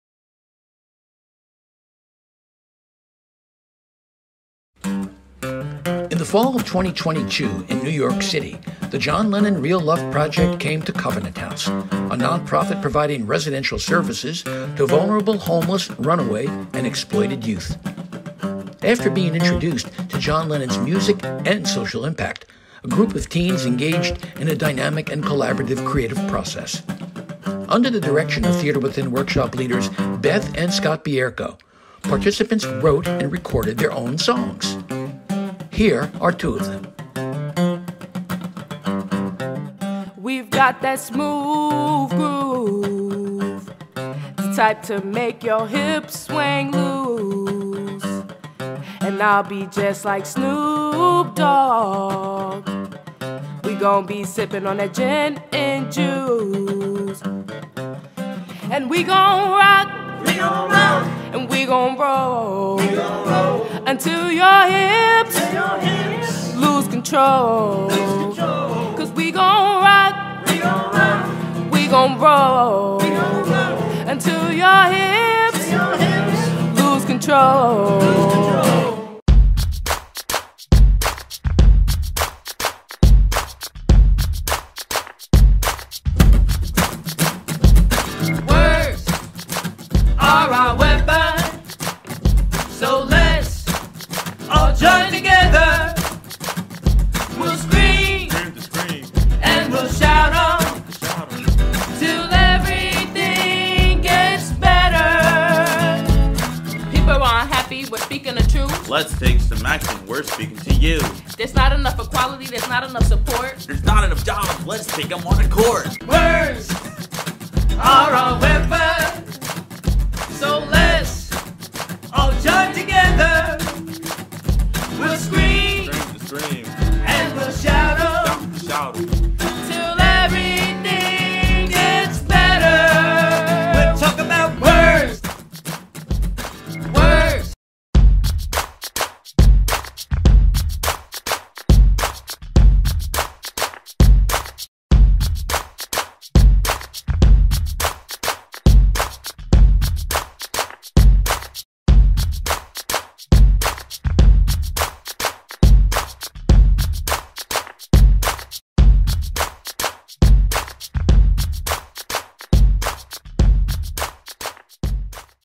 The John Lennon Real Love Project at Covenant House, NY
The John Lennon Real Love Project, provided by Theatre Within, is a visionary songwriting program for communities in need. These recordings, under the direction of Beth and Scott Bierko, were made October 5 - 7, 2022.